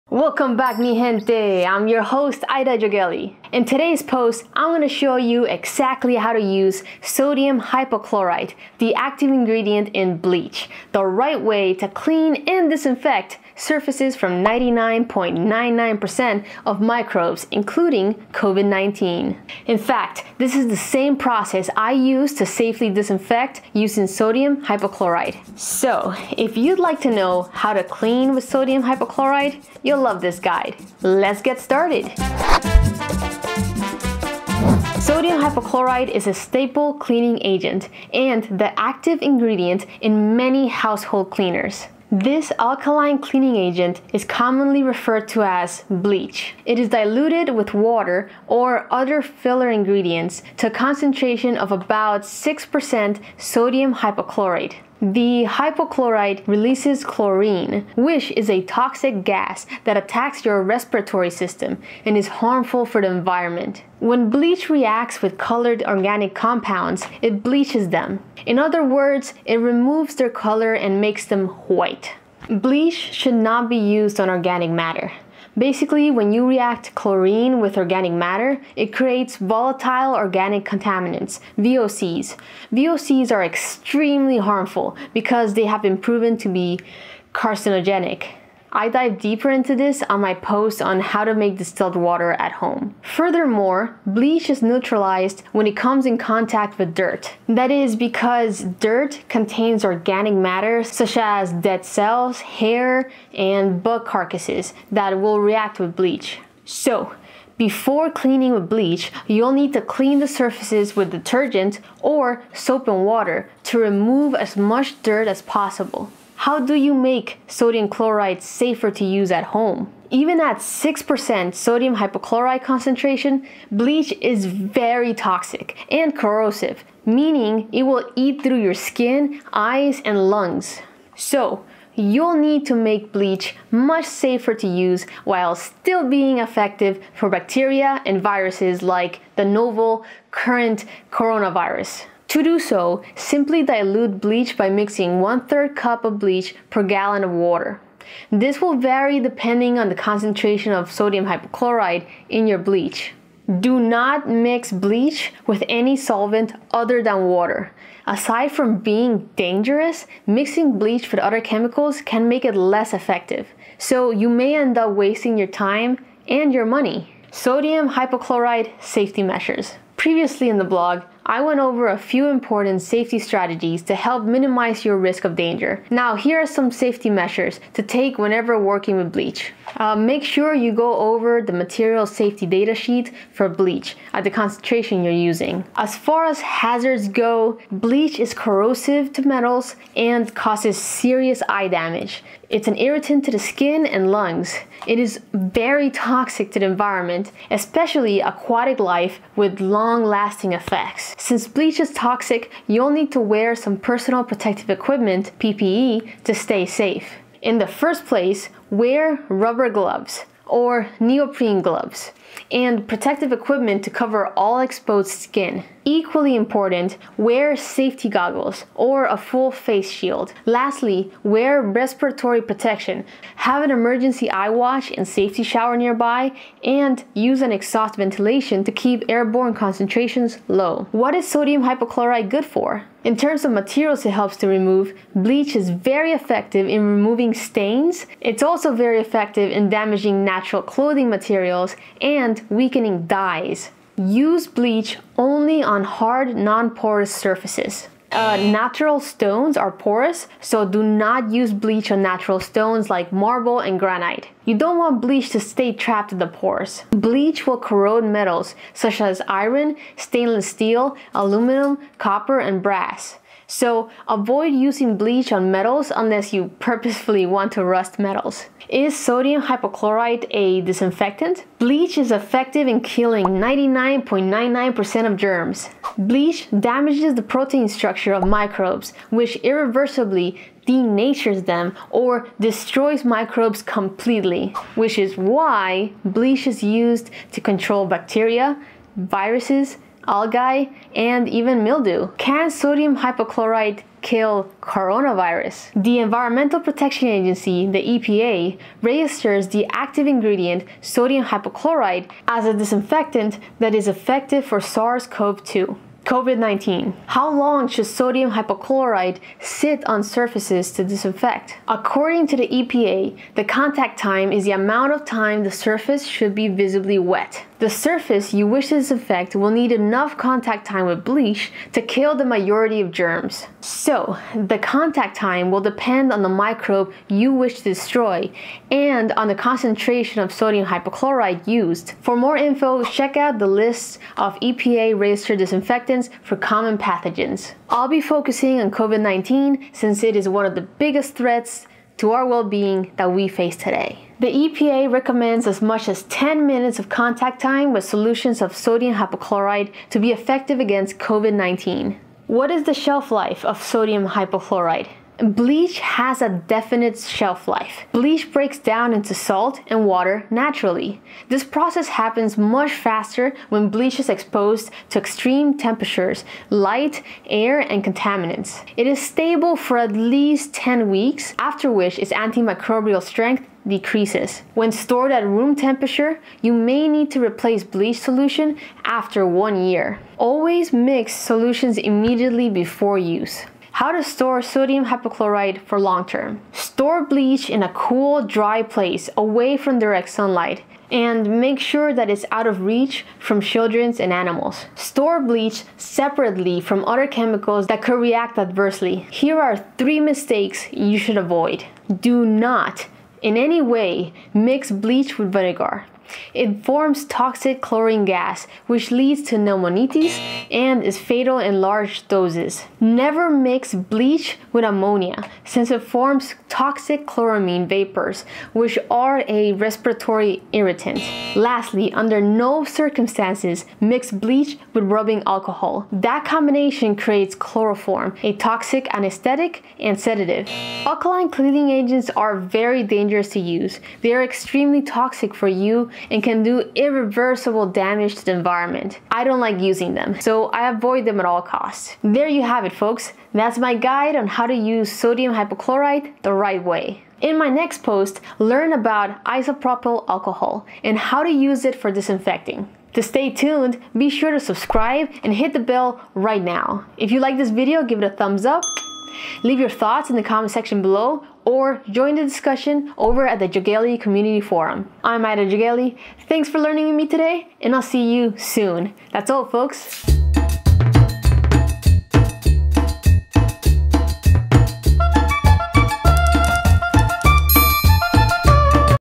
How To Use Bleach Disinfectant
I teach you how to use bleach the right way to disinfect surfaces from 99.99% of microbes.

Use sodium hypochlorite, the active ingredient in bleach, to clean the right way and you can get rid of bacteria and viruses. In fact, this is the same process I use to safely disinfect using sodium hypochlorite.

So if you would like to know “How to clean with sodium hypochlorite?”, you’ll love this guide.
Visit the CDC´s Site to get the latest information about the Coronavirus Disease
0:34 - How to Use Sodium Hypochlorite (Bleach) the Right Way
1:56 - How Do You Make Sodium Hypochlorite Safer to Use at Home?
2:47 - Sodium Hypochlorite Safety Measures
3:49 - What is Sodium Hypochlorite Good For?
4:29 - Is Sodium Hypochlorite a Disinfectant?
4:50 - Can Sodium Hypochlorite Kill viruses?
5:06 - How Long Should Sodium Hypochlorite Sit on Surface to Disinfect?
5:50 - What is the Shelf Life of Sodium Hypochlorite?
6:23 - How to Store Sodium Hypochlorite for Long Term?